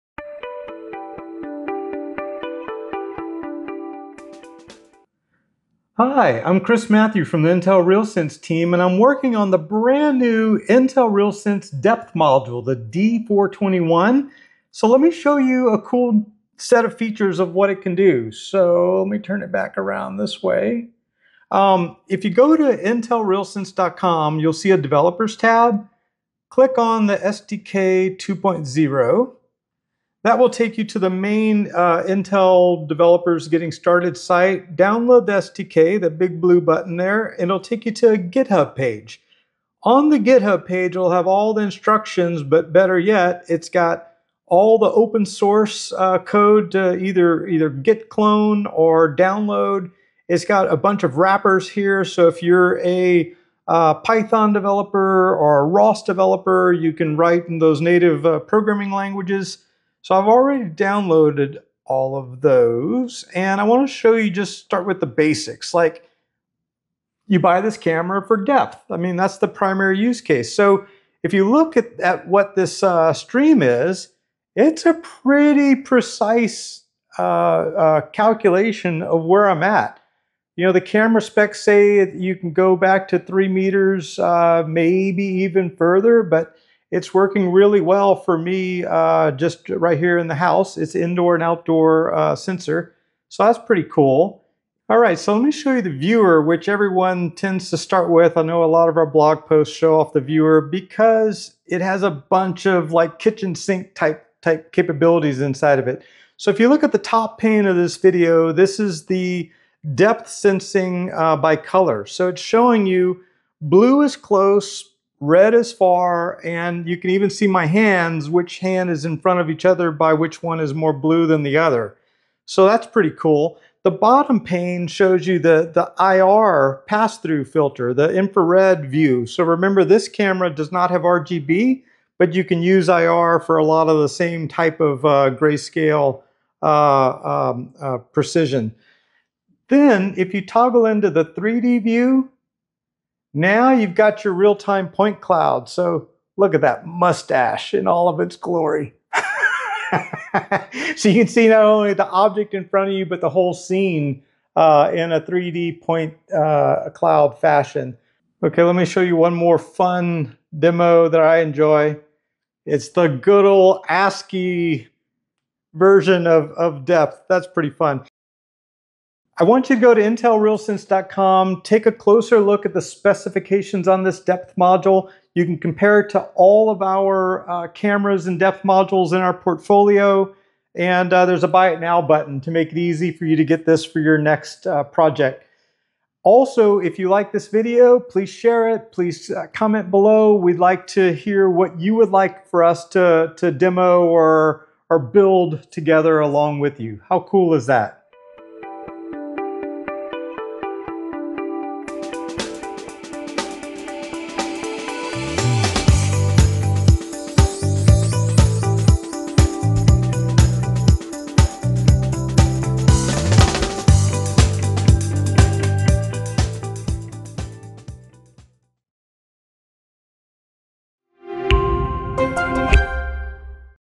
D421 Setup
We show you how to setup the Intel® RealSense™ Depth Module D421. Designed as an entry-level solution, this all-in-one stereo depth module is equipped with the powerful D4 Vision Processor and an integrated optical module – performing all the depth calculations onboard. Whether it's for Autonomous Mobile Robots (AMRs) in dynamic environments like restaurants, retail spaces, or hospitals,  the Intel RealSense Depth Module D421 delivers advanced depth sensing technology tailored for a wider audience at an affordable price point.